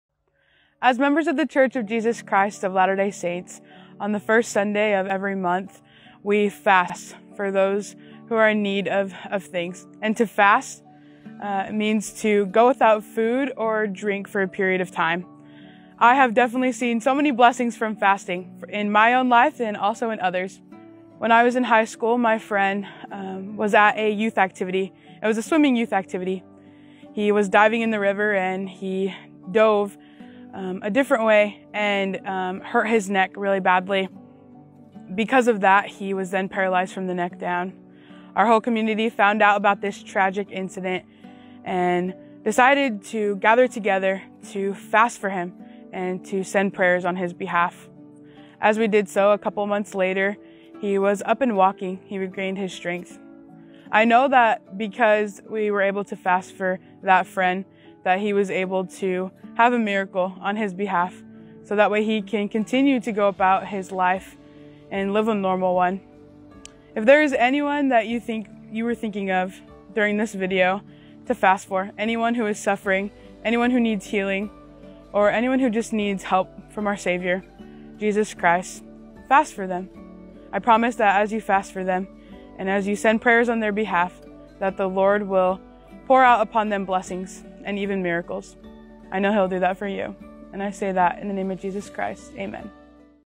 The Power Of Fasting
Fasting means going without food for a period of time as a sacrifice to the Lord. Fasting, when united with prayer, is a powerful way to access help from heaven! Sometimes, fasting prepares us to witness miracles in our lives; other times, fasting prepares our heart to accept the Lord's will and recognize his hand already in our life. When we fast, we are allowing God's will to prevail in our life, whatever His desires and timing for us are. Listen to Sister Bri Molina as she describes a time when fasting and prayer helped her friend who was seriously injured.

If you would like to learn more about fasting, and how it can bless your life in a variety of ways, click here!